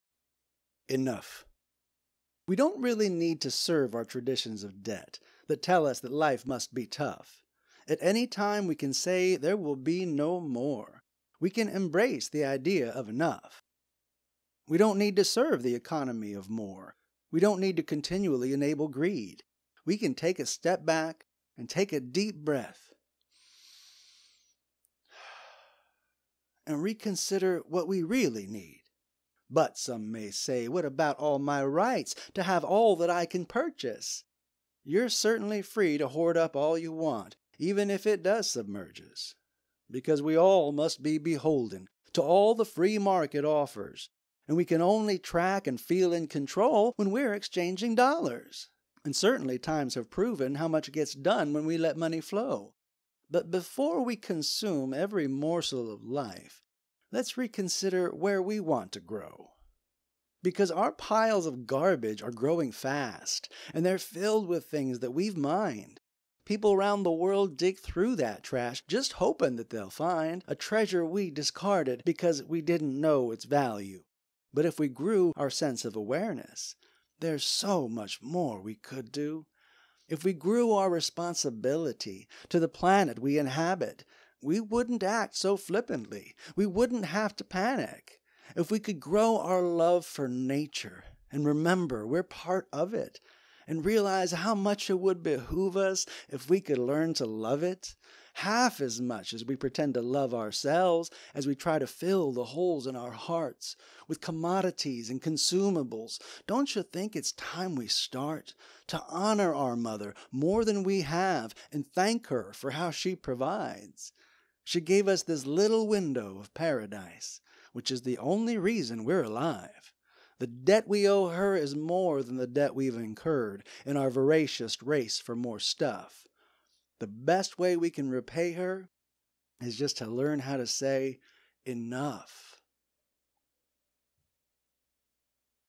Enough
"Enough" - a poem by Steve McAllister

we don't really need to serve our traditions of debt
that tell us that life must be tough
at any time we can say there will be no more
we can embrace the idea of enough
we don't need to serve the economy of more
we don't need to continually enable greed
we can take a step back and take a deep breath
and reconsider what we really need
but, some may say, what about all my rights
to have all that I can purchase
you're certainly free to hoard up all you want
even if it does submerge us
because we all must be beholden
to all the free market offers
and we can only track and feel in control
when we're exchanging dollars
and certainly times have proven
how much gets done when we let money flow
but before we consume every morsel of life
let's reconsider where we want to grow
because our piles of garbage are growing fast
and they're filled with things that we have mined
people 'round the world dig through that trash
just hoping that they'll find
a treasure we discarded
because we didn't know its value
but if we grew our sense of awareness
there's so much more we could do
if we grew our responsibility
to the planet we inhabit
we wouldn't act so flippantly
we wouldn't have to panic
if we could grow our love for nature
and remember we're part of it
and realize how much it would behoove us
if we could learn to love it
half as much as we pretend to love ourselves
as we try to fill the holes in our hearts
with commodities and consumables
don't you think it's time we start
to honor our mother more than we have
and thank her for how she provides
she gave us this little window of paradise
which is the only reason we're alive
the debt we owe her is more than the debt we've incurred
in our voracious race for more stuff
the best way we can repay her
just to learn how to say "enough"